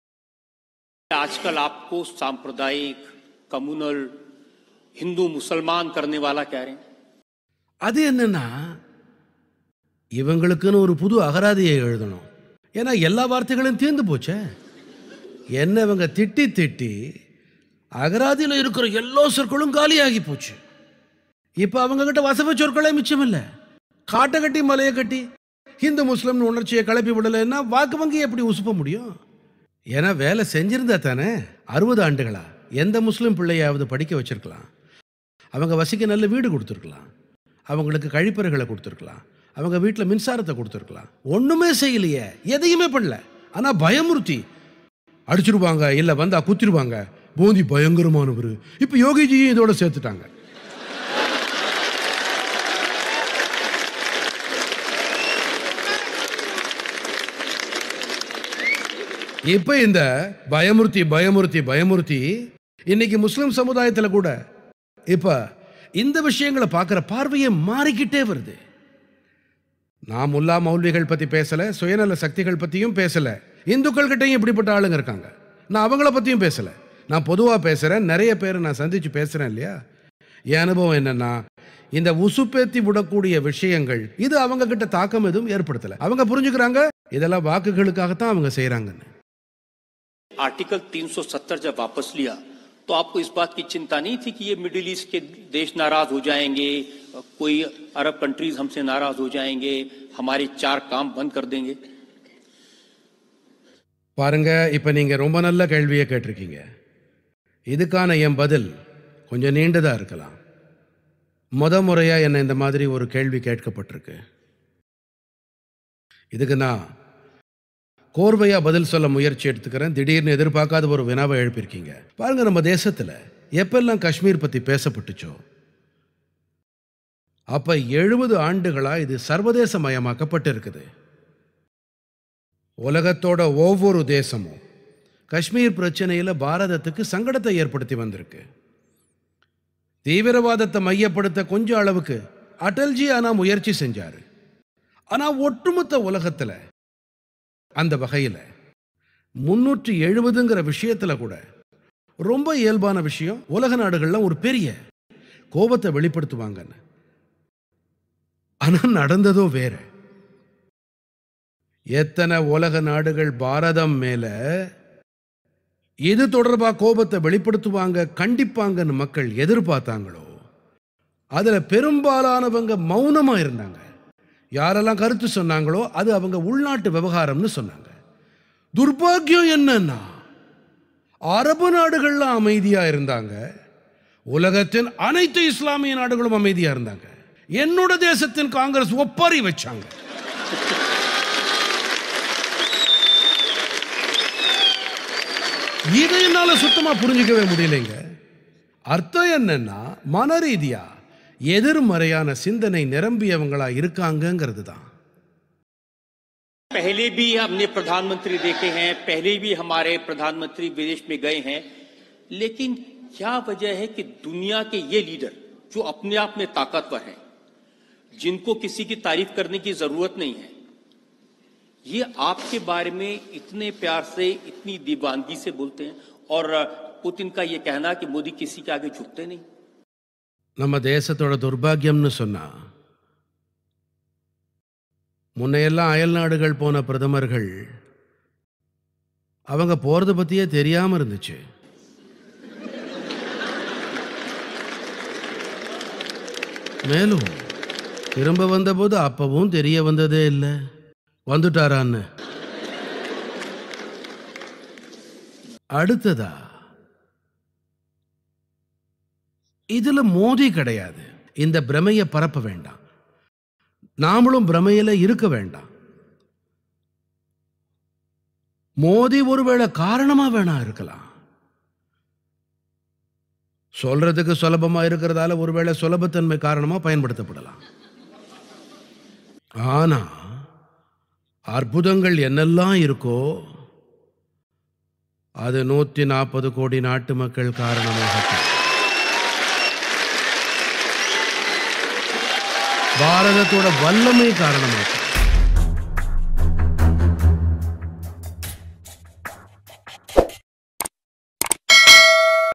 என்னைக் காட்டி முஸ்லிம்களை பயமுறுத்தினார்கள், இப்போது யோகியைக் காட்டுகிறார்கள் : பிரதமர் மோடி
என்னைக் காட்டி முஸ்லிம்களை பயமுறுத்தினார்கள், இப்போது யோகியைக் காட்டி பயமுறுத்துகிறார்கள் என்கிறார் பிரதமர் மோடி. மேலும் சட்டப்பிரிவு 370 நீக்கத்தின் போது முஸ்லிம் அரபு நாடுகள் கூட அமைதியாக இருந்தன. ஆனால் காங்கிரஸ் குதித்தது என்கிறார்.